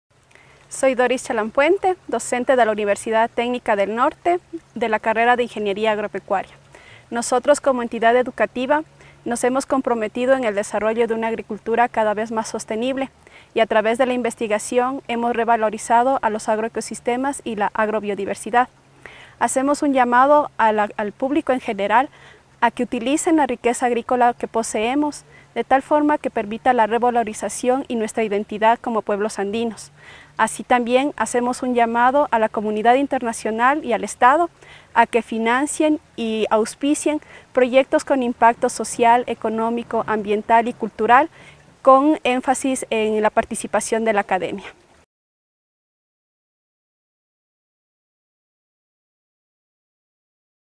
Ms Chalampuente: ‘We are committed to develop increasingly sustainable agriculture’
This video is part of the ‘Voices of Diversity’ series, with testimonials from people from over 25 countries, who foster plant genetic diversity and play active roles in the International Plant Treaty’s Benefit-sharing Fund.

Note: All messages were written and recorded by the participants themselves, consciously taking the opportunity to share their insights, successes and requests.